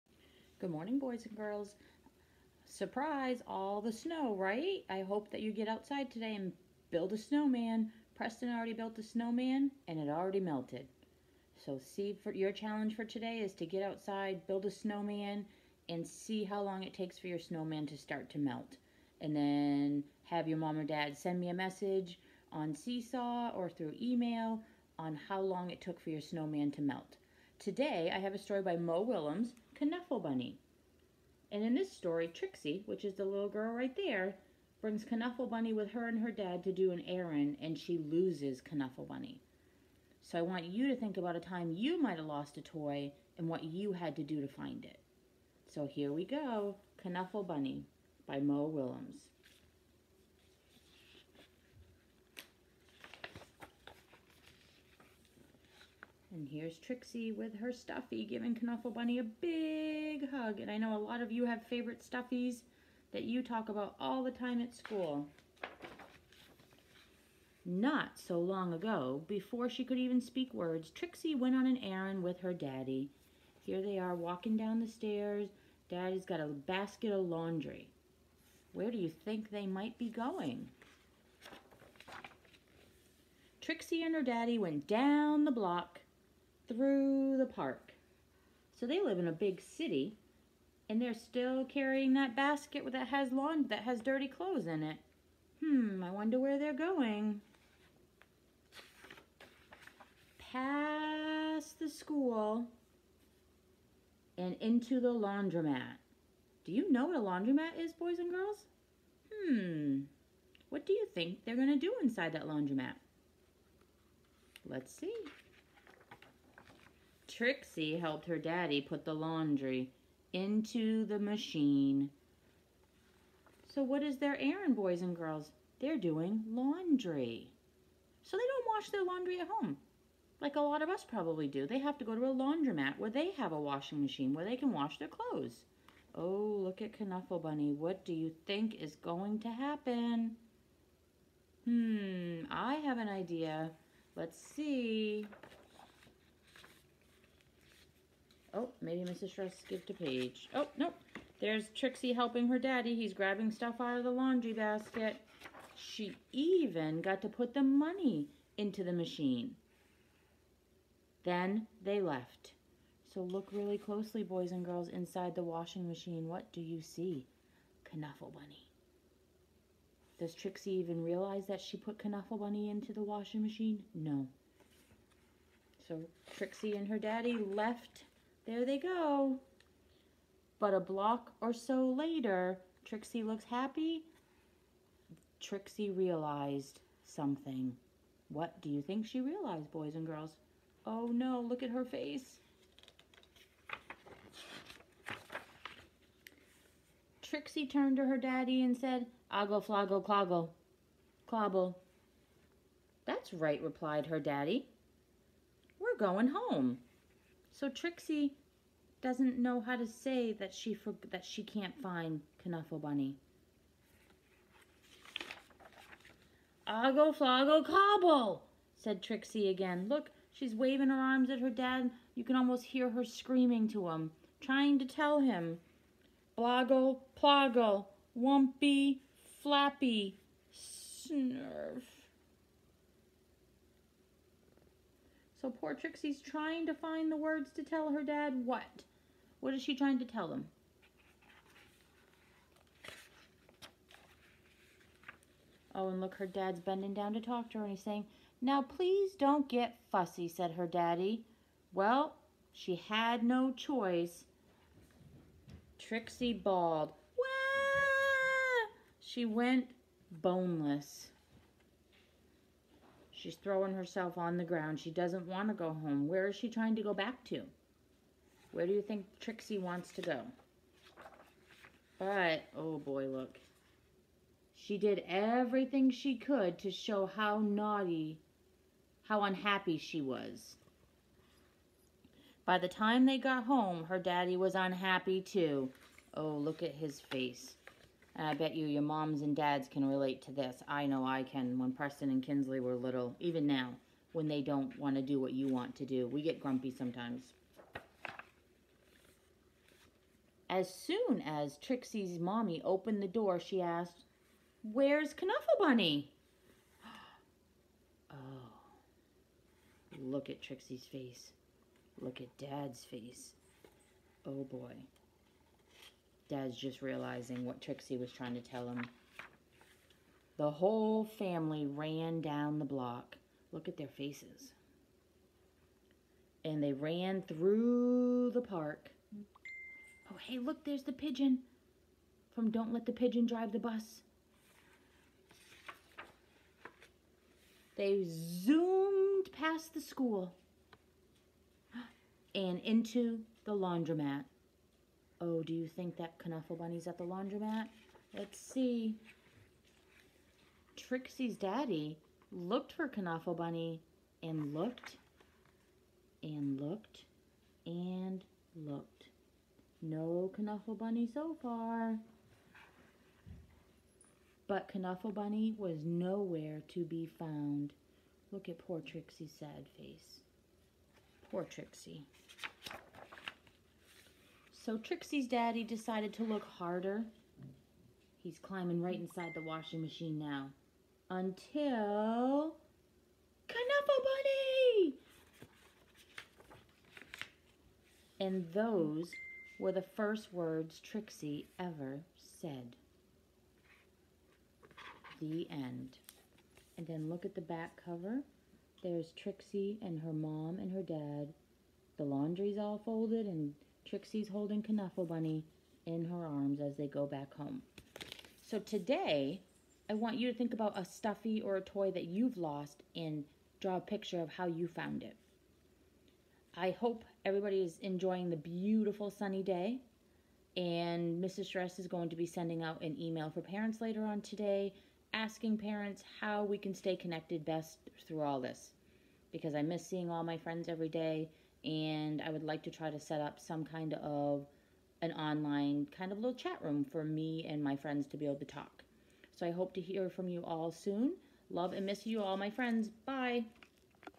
Knuffle Bunny by Mo Willems
Mrs. Charest reads Knuffle Bunny